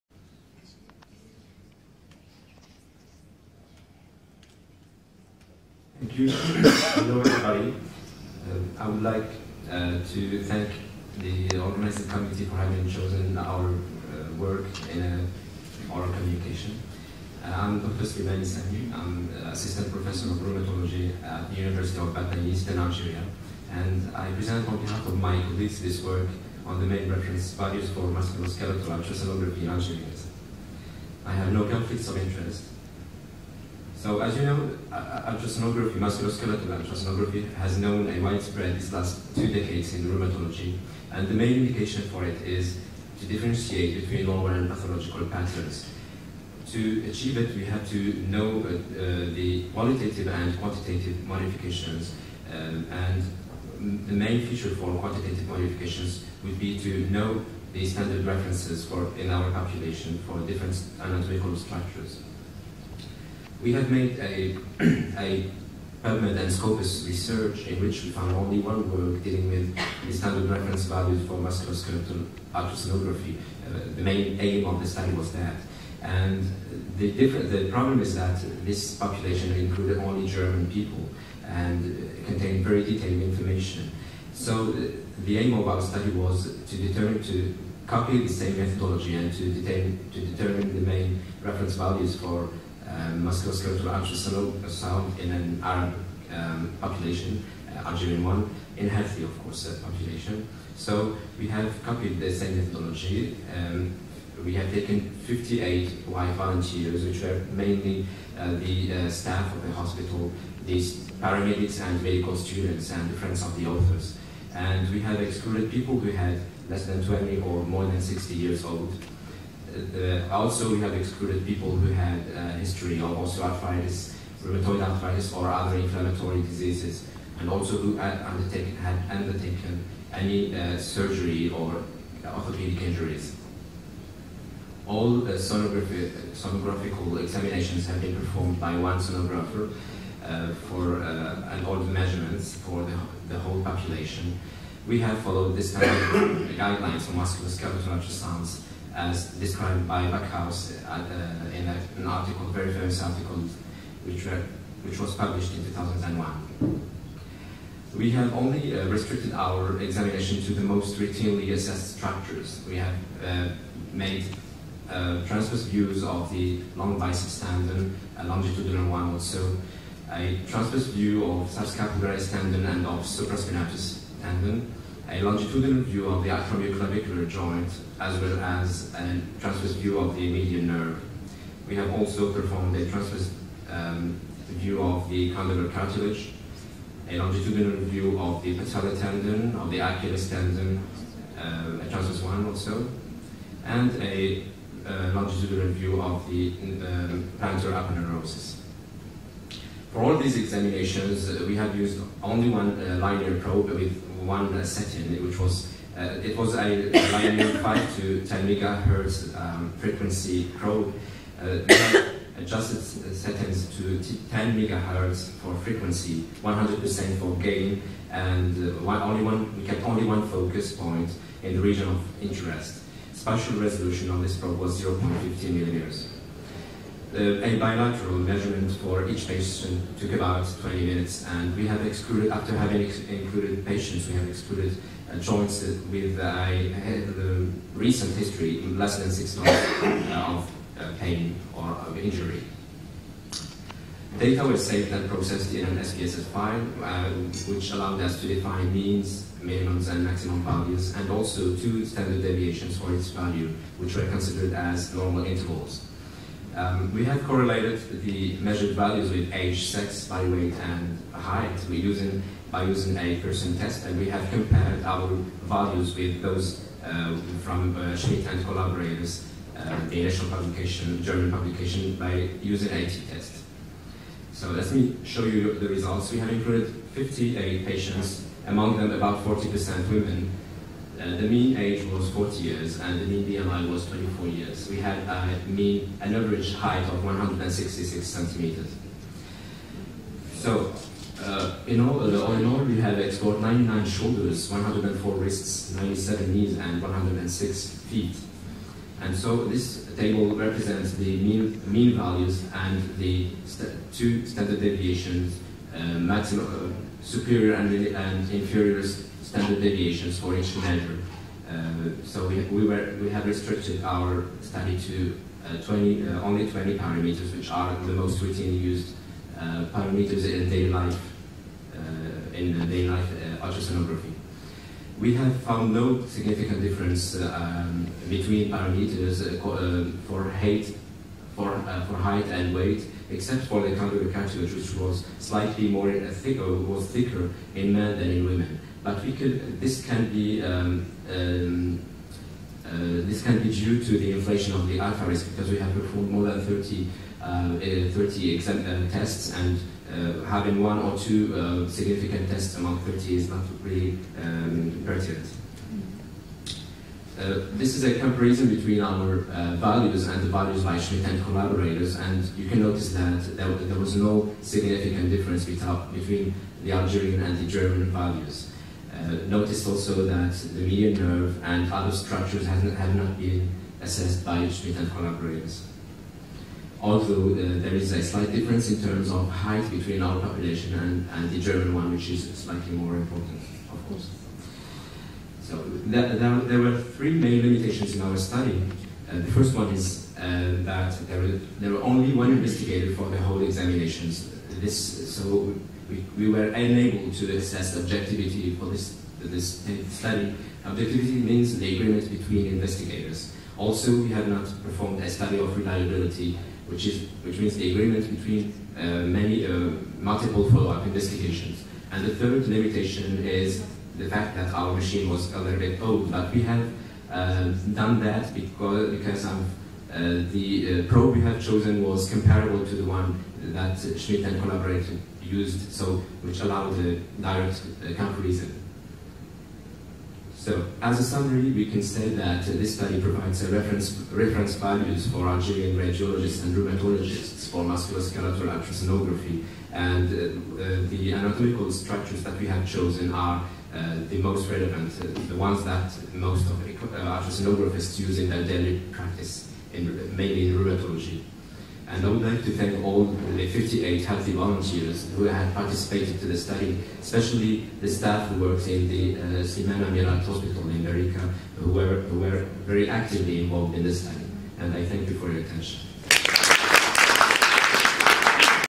Communication Slimani
Communication Dr. Slimani lors du congrès Panarabe de rhumatologie. Intitulé : "Main Reference Values for Musculoskeletal Ultrasonography in Algerians"